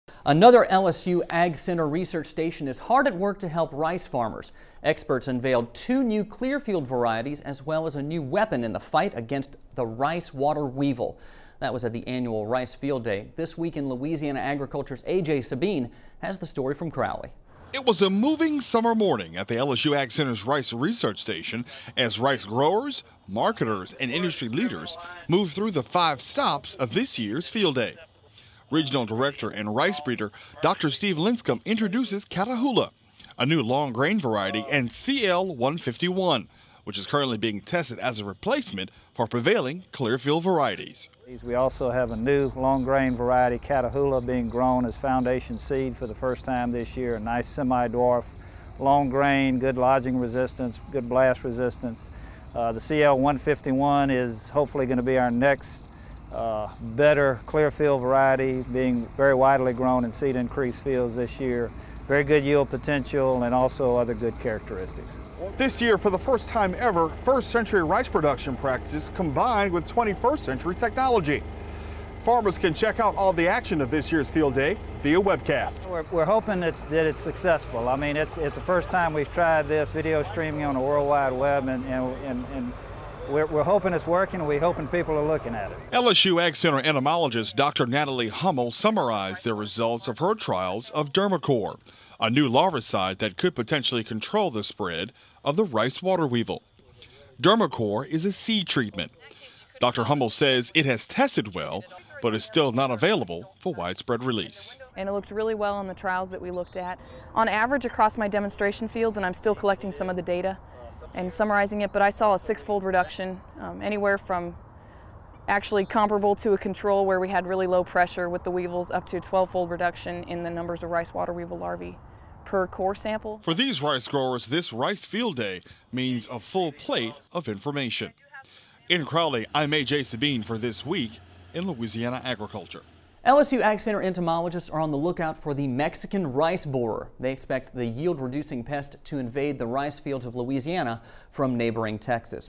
Louisiana Farm Bureau: Crowley Rice Field Day
This Week in Louisiana Agriculture's A.J. Sabine takes us to Crowley, LA where the LSU AgCenter's Rice Research Station unveiled some new varieties of rice.